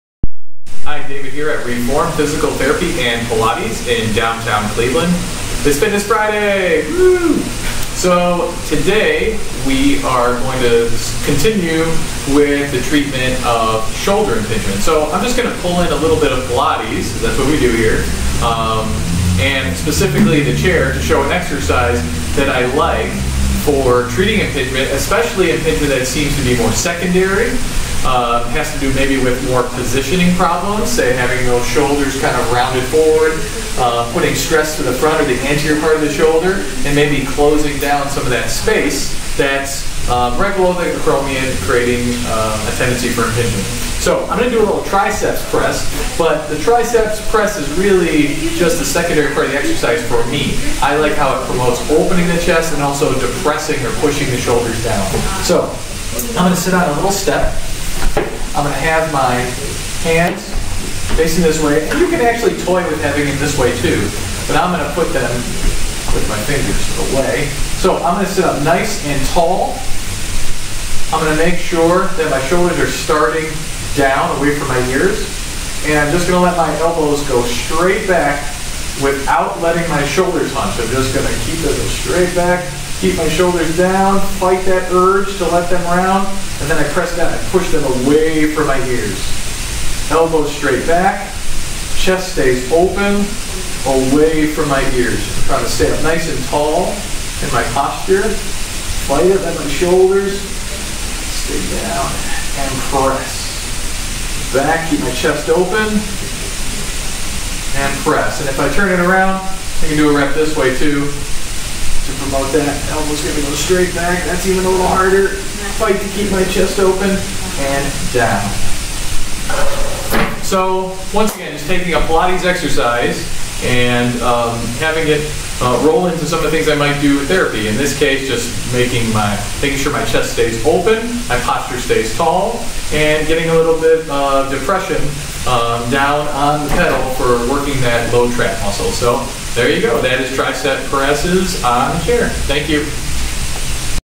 Wunda Chair Tricep Press
We are finishing up our Shoulder Impingement series this week with a Pilates exercise that is great for those who are dealing with shoulder impingement. This exercise will work the lower trap muscle to promote better muscle coordination.